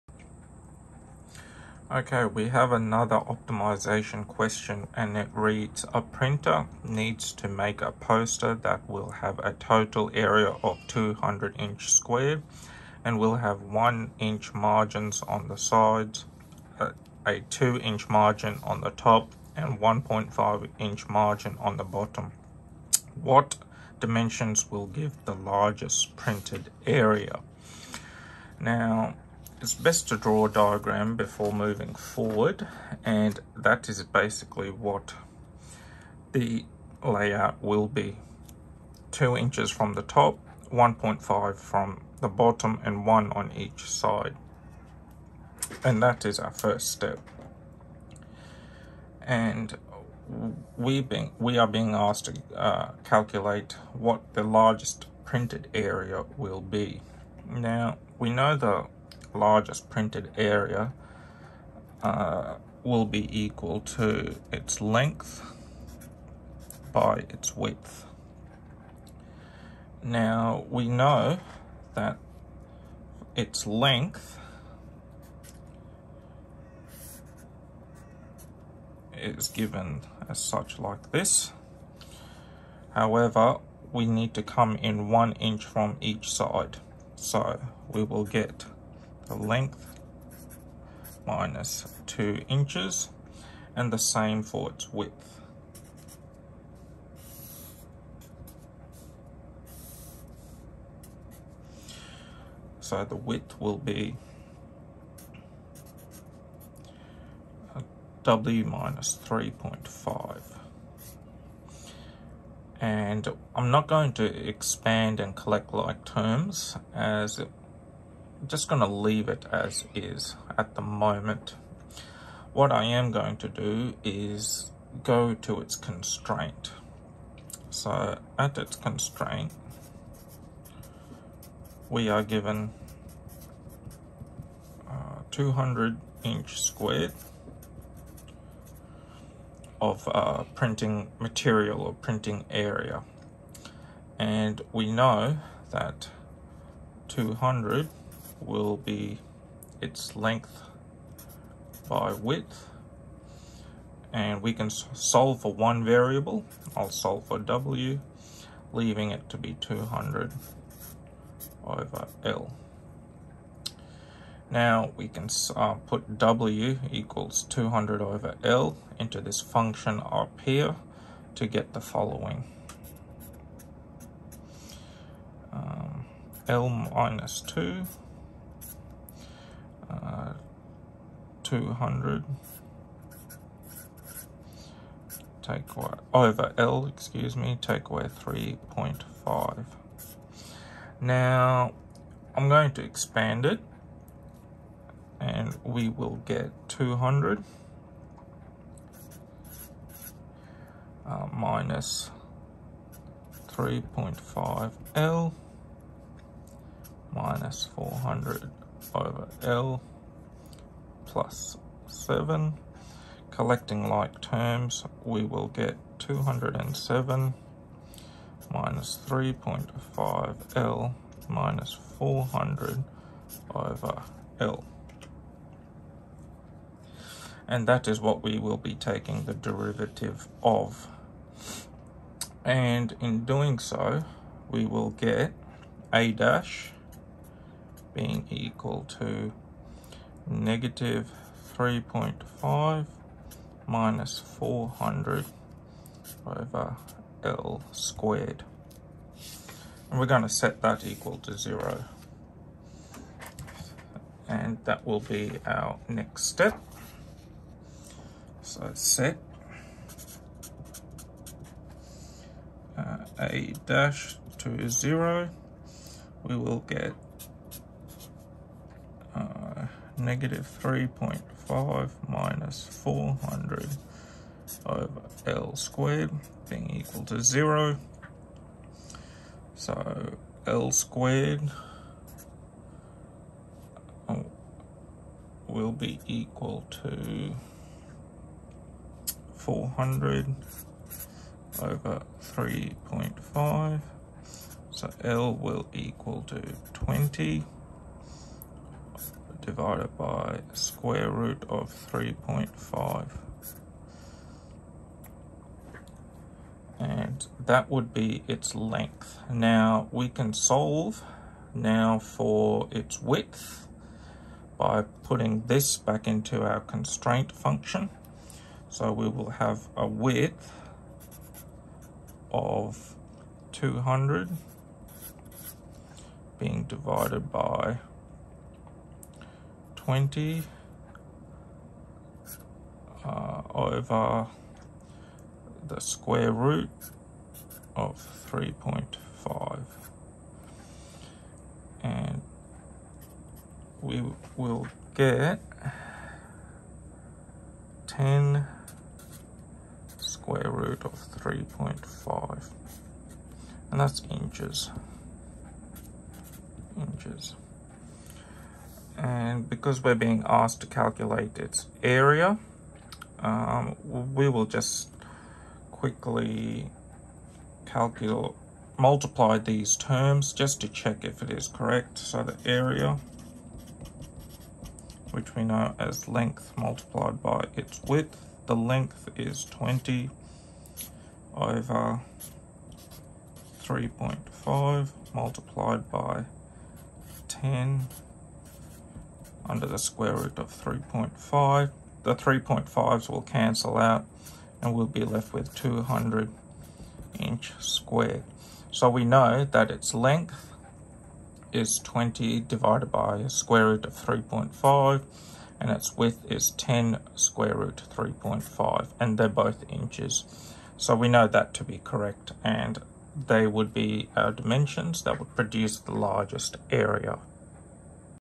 Optimization Question - Printed Poster
A printer needs to make a poster that will have a total area of 200𝑖𝑛2 and will have 1 inch margins on the sides, a 2 inch margin on the top and a 1.5 inch margin on the bottom. What dimensions will give the largest printed area?